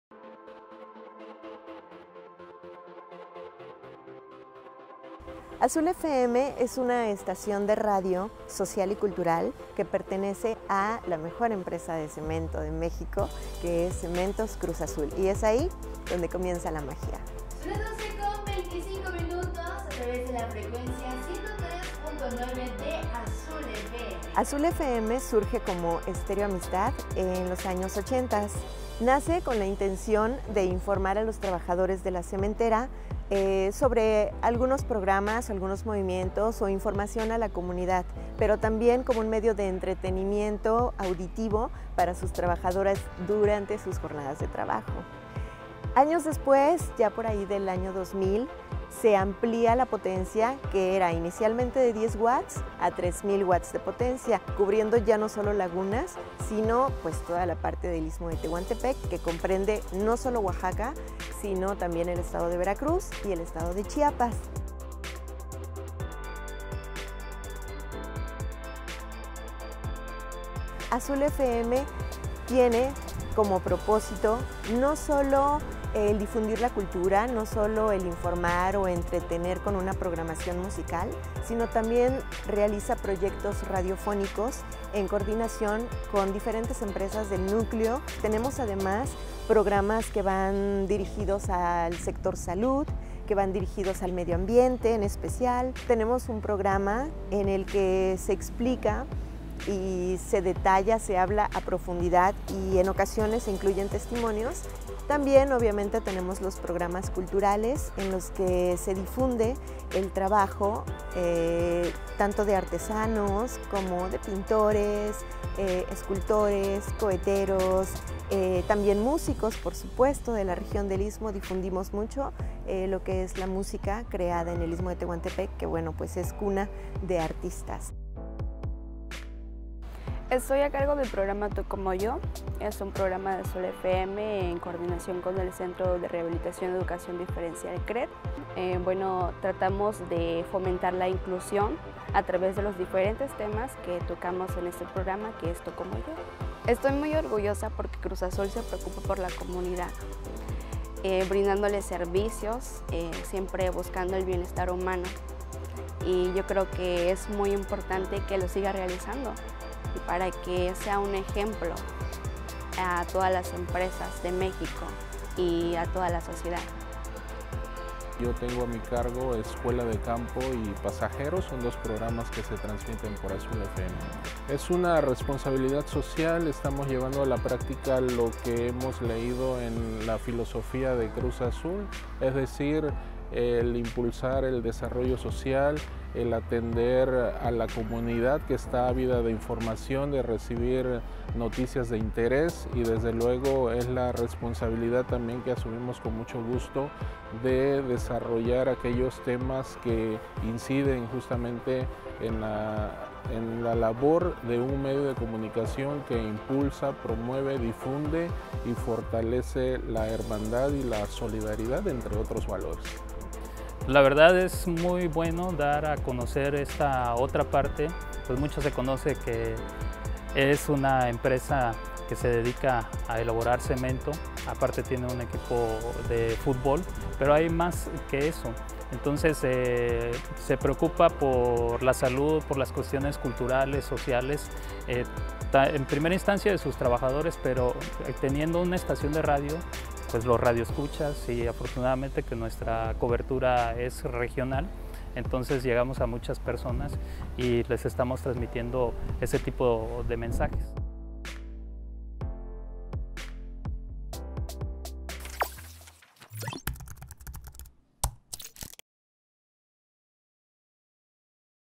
¿Qué es Azul FM?
Cruz Azul se distingue por sus prácticas responsables, entre ellas es la estación de radio con la que cuenta en Lagunas, Oaxaca. ¿Quieres sabes más de este esfuerzo responsable? ¡Dale play a este video!